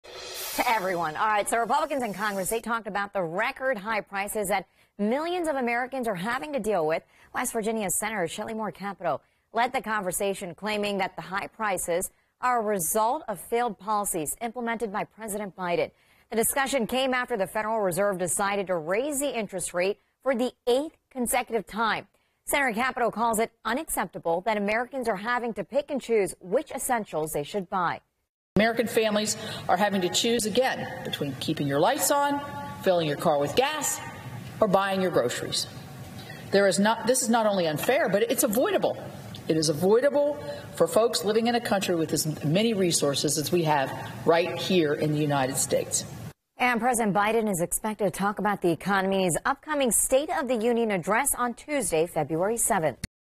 WDVM: Capito Speaks on the Middle Class Squeeze
U.S. Senator Shelley Moore Capito (R-W.Va.) led her Republican colleagues in drawing attention to the middle class squeeze being felt due to President Biden's failed economic policies.